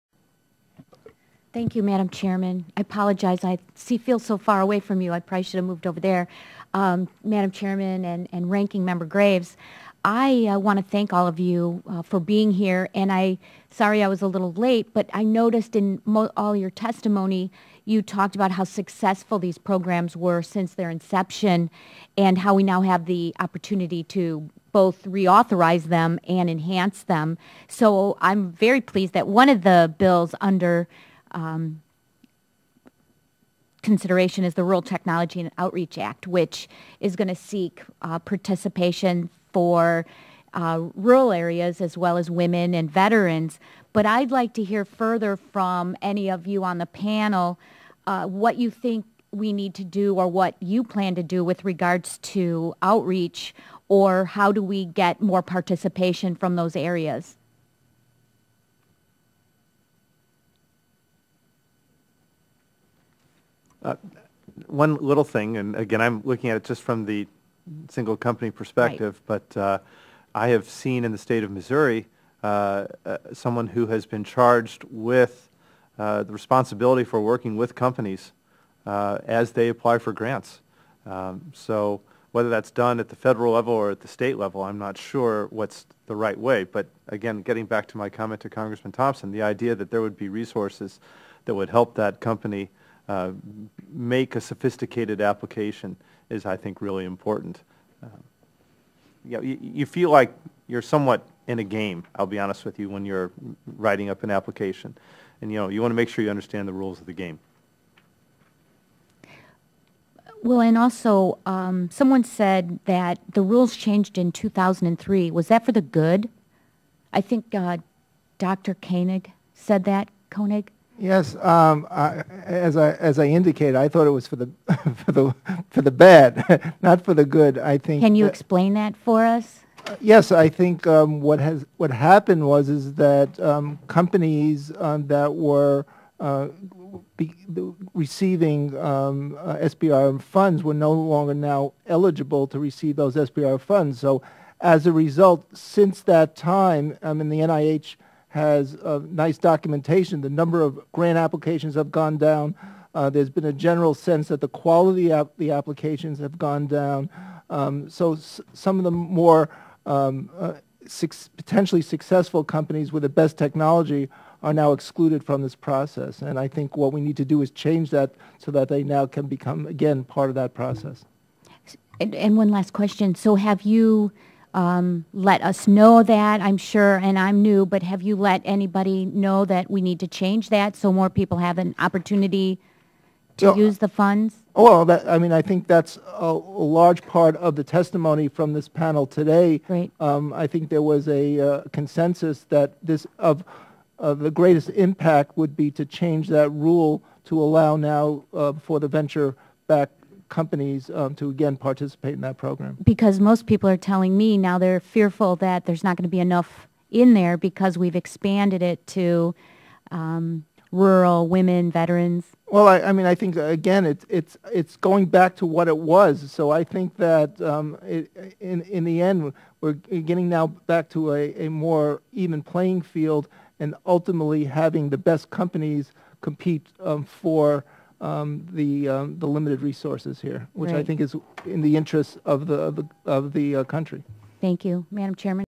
Rep Halvorson D IL Question and Answer with Panel
Question-and-answer session conducted by Rep. Debbie Halvorson (D-IL) at the House Small Business Committee hearing on Legislative Initiatives to Strengthen and Modernize the SBIR and STTR Programs on June 17, 2009